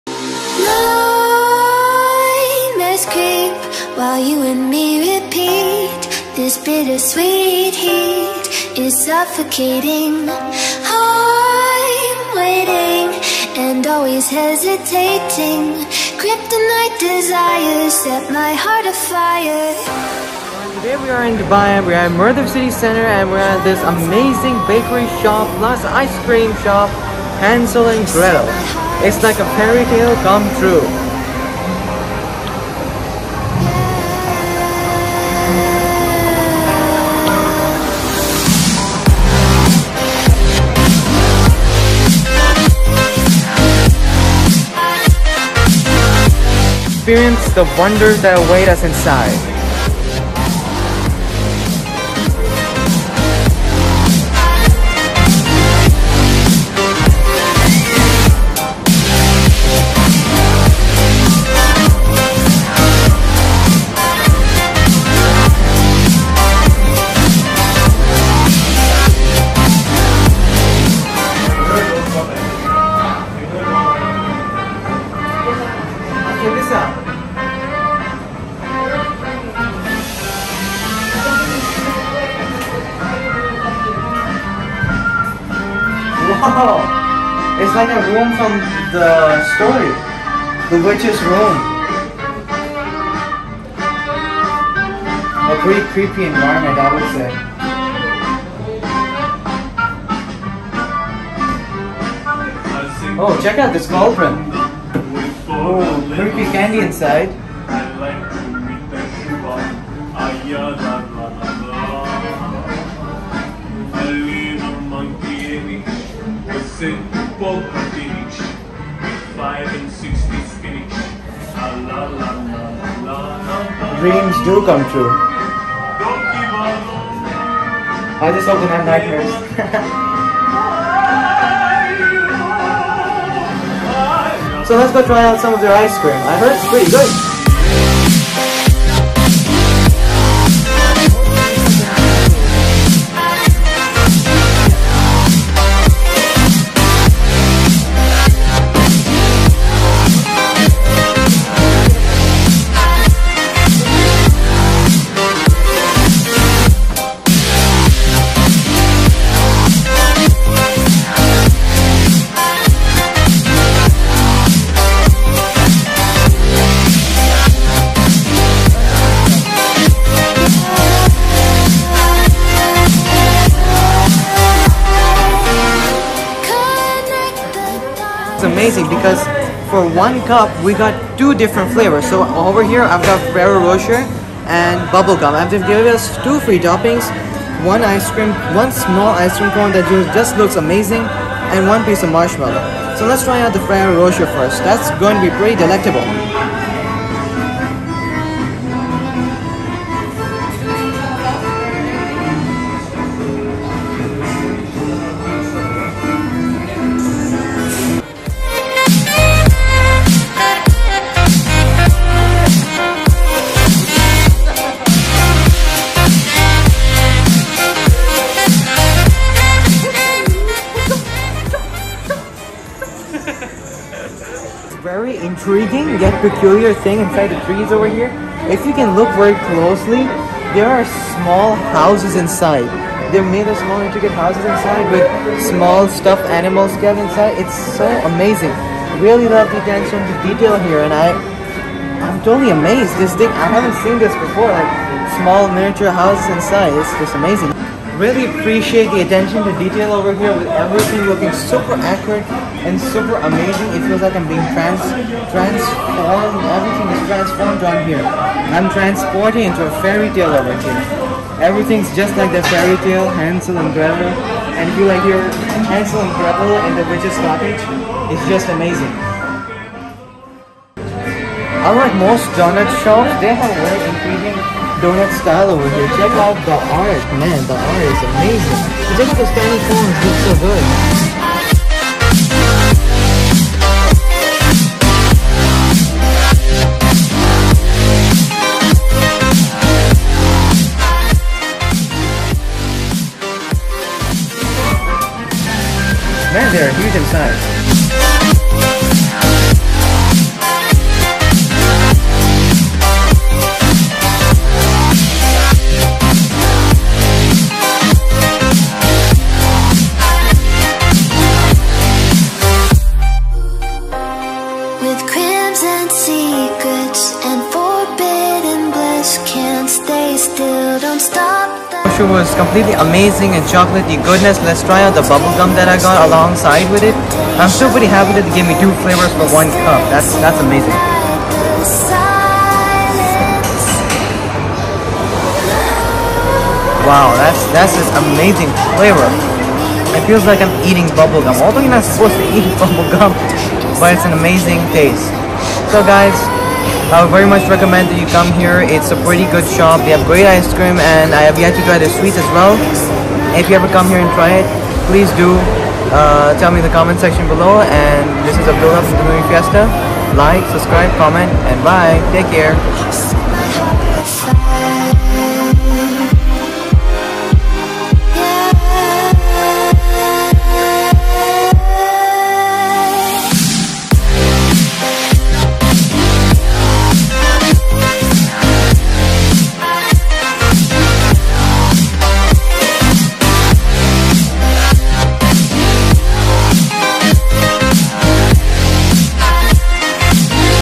Fairy Tale Hans and Gretel: Sweets and Ice cream shop
Have you ever wanted to live in a fairy tale , just for a brief moment of your life , then look no further than the spectacular Hans and Gretel sweet shop in Dubai , this Hansel and Gretel fairy tale themed venue transport you into the magical world of sweets, ice cream and imagination , so come along with me and check it out